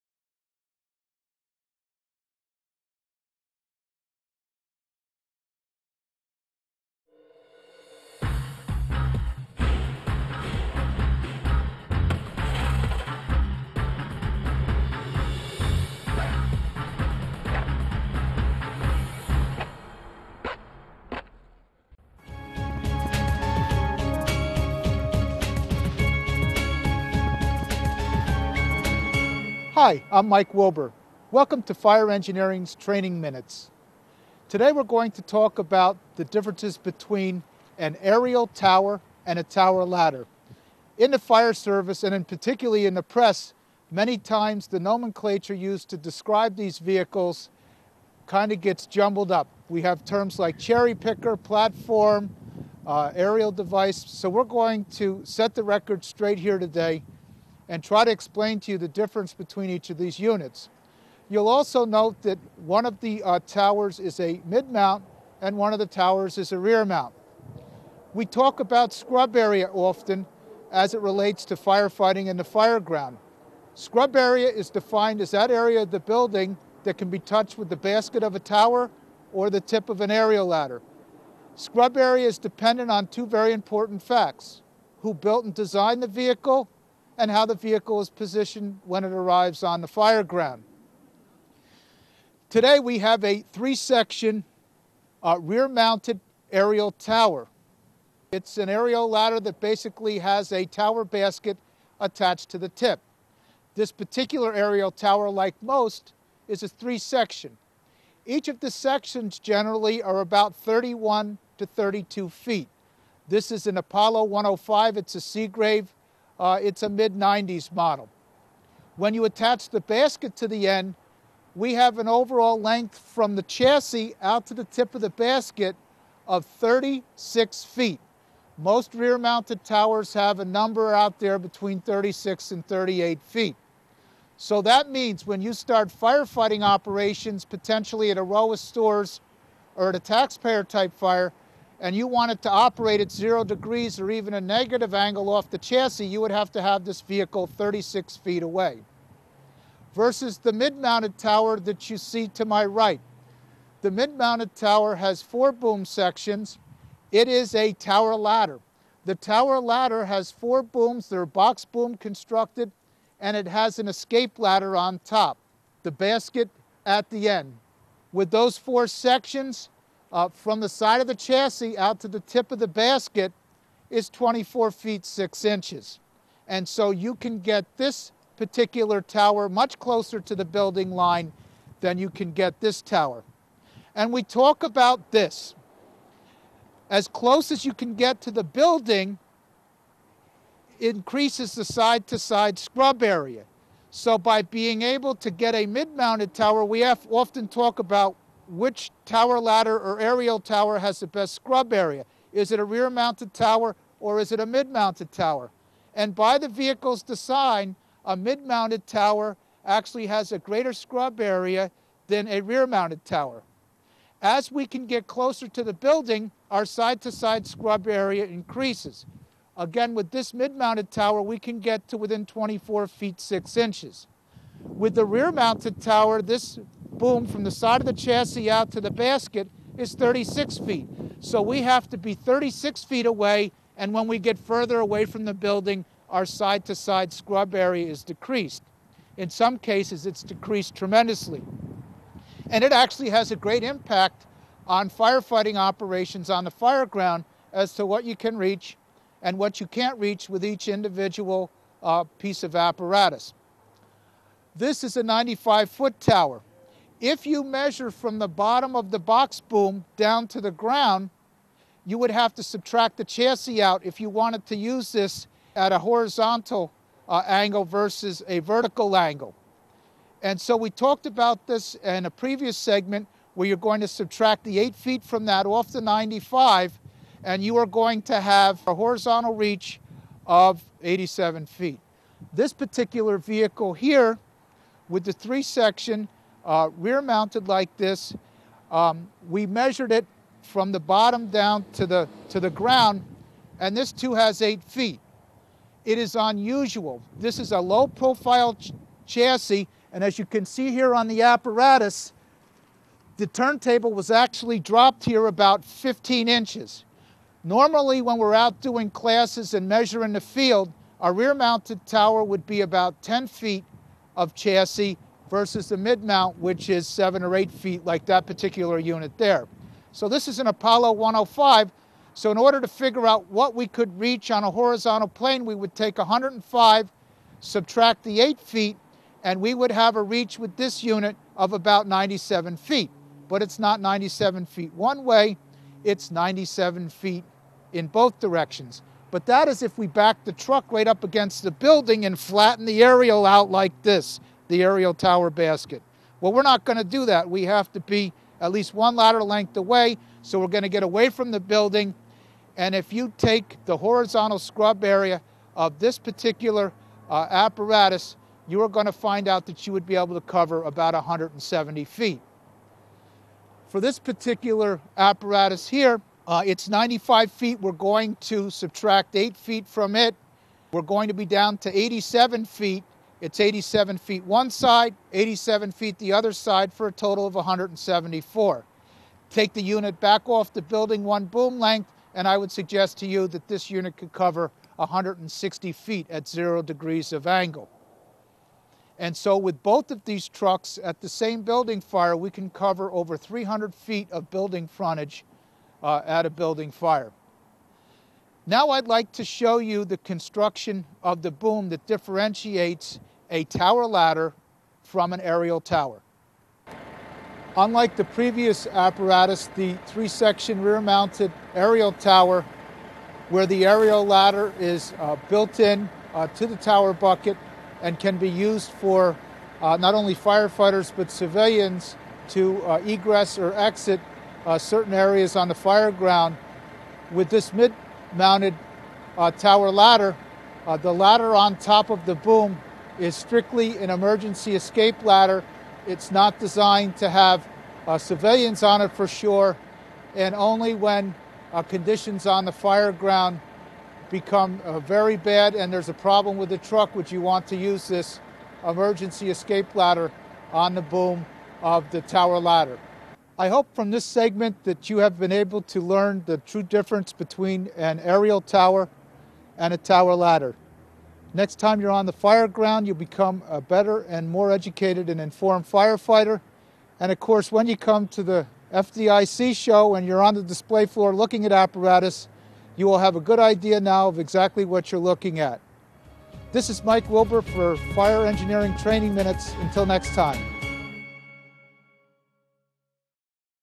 Aerial Tower vs. Tower Ladder #1380771835001
Mike Wilbur discusses the operational differences between an aerial tower and tower ladder.